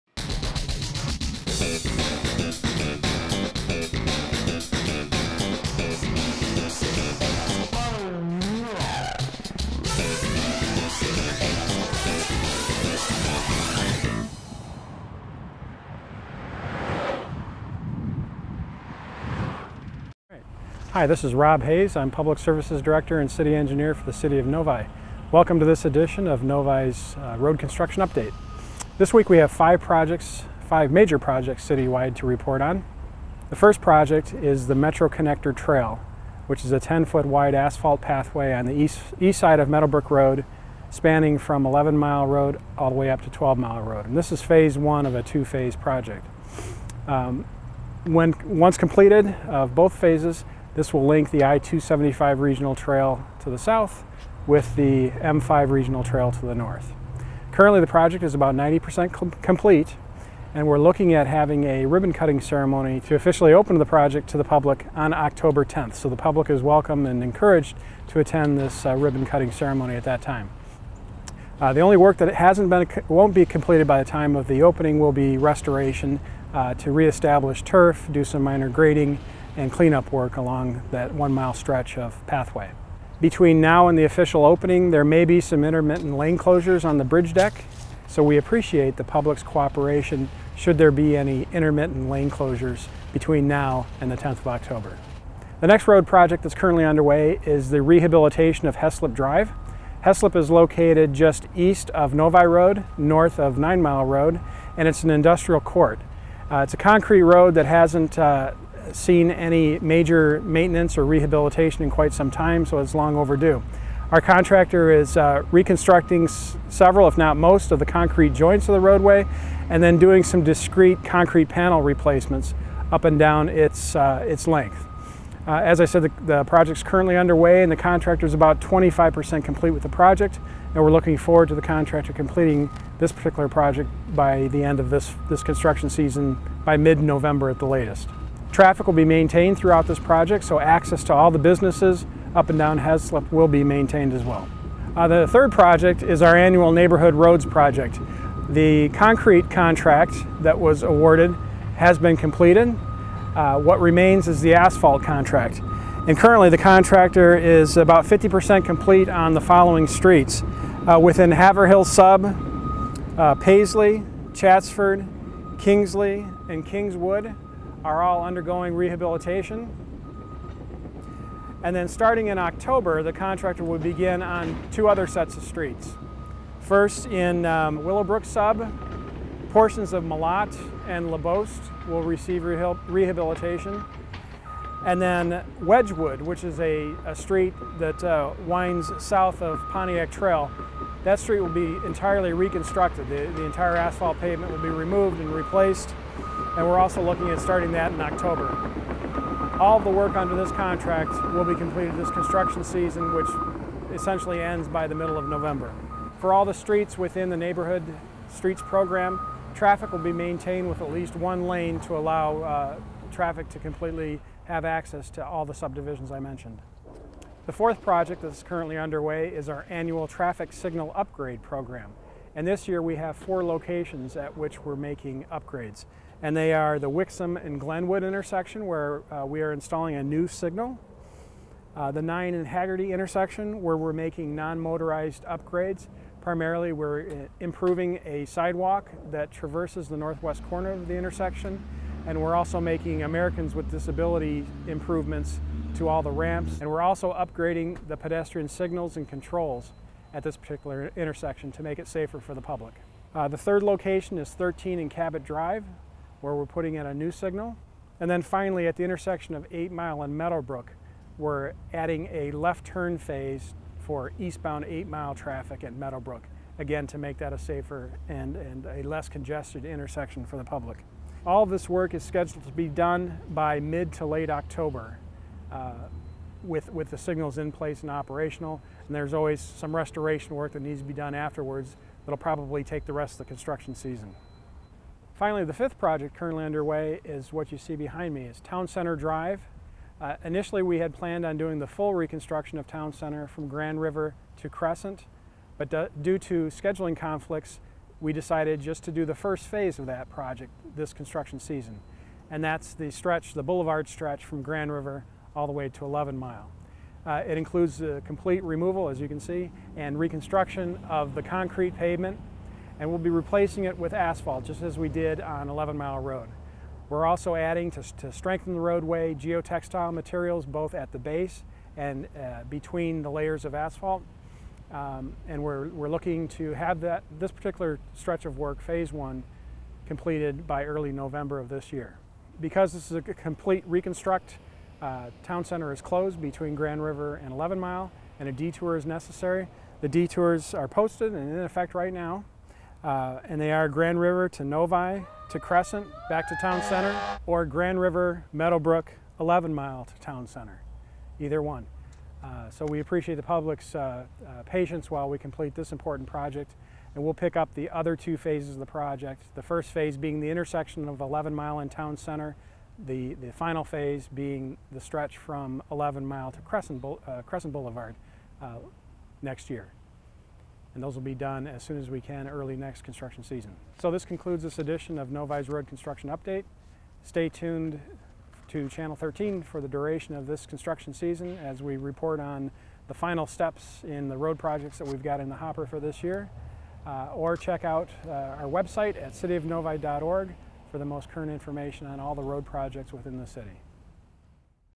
Construction Update: September/October
Current Projects
- Town Center Drive Reconstruction
- 2014 Traffic Signal Improvements
- Heslip Drive Rehabilitation
- 2014 Neighborhood Road Program
- Metro Connector Trail